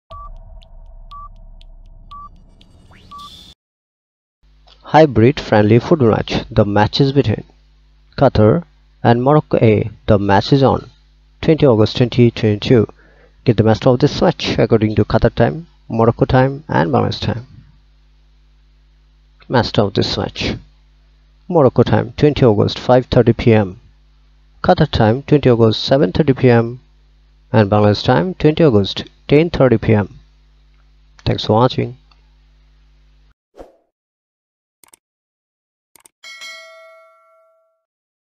Qatar vs Morocco A - Hybrid Friendly Football Match - 20 August 2022
Qatar and Morocco A football teams are meeting in Hybrid Friendly Football Match on 20 August 2022.

Get the match time of this match according to Qatar, Morocco, and Bangladesh time.